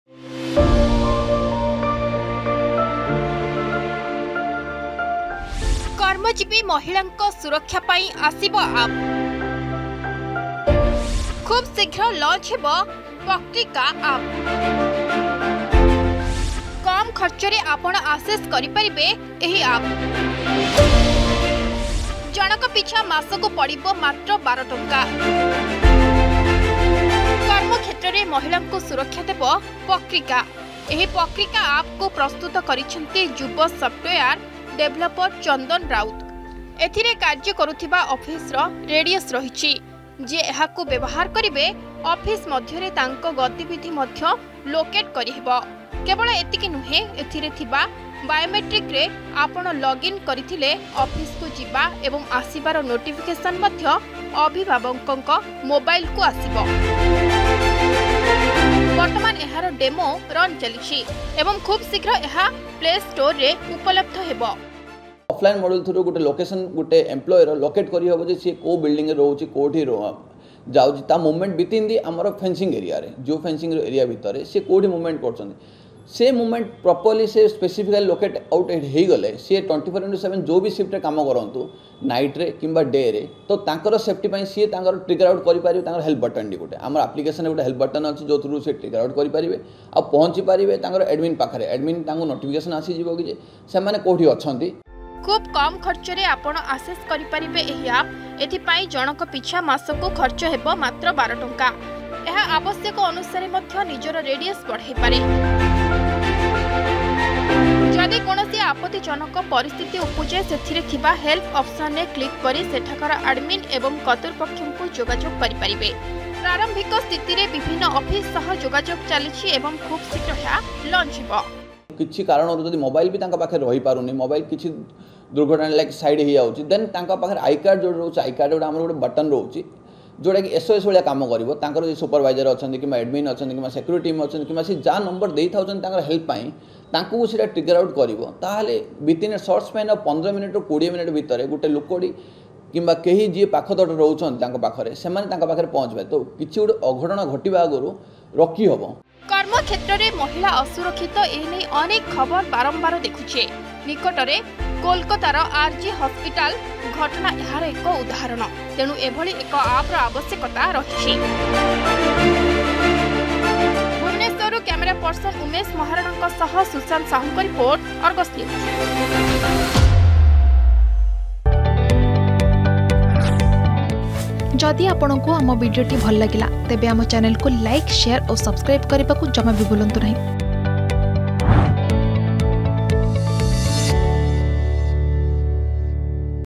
କର୍ମଜୀବୀ ମହିଳାଙ୍କ ସୁରକ୍ଷା ପାଇଁ ଆସିବ ଆପ୍ || Pakrika Aplications || Working Women
ଖୁବଶୀଘ୍ର ଲଞ୍ଚ ହେବ `ପକ୍ରିକା' ଆପ୍। କମ୍ ଖର୍ଚ୍ଚରେ ଆପଣ ଆସେସ କରିପାରିବେ ଏହି ଆପ୍ । ଜଣକ ପିଛା ମାସକୁ ପଡ଼ିବ ମାତ୍ର ୧୨ ଟଙ୍କା । କର୍ମ କ୍ଷେତ୍ରରେ ମହିଳାଙ୍କୁ ସୁରକ୍ଷା ଦେବ ପକ୍ରିକା ।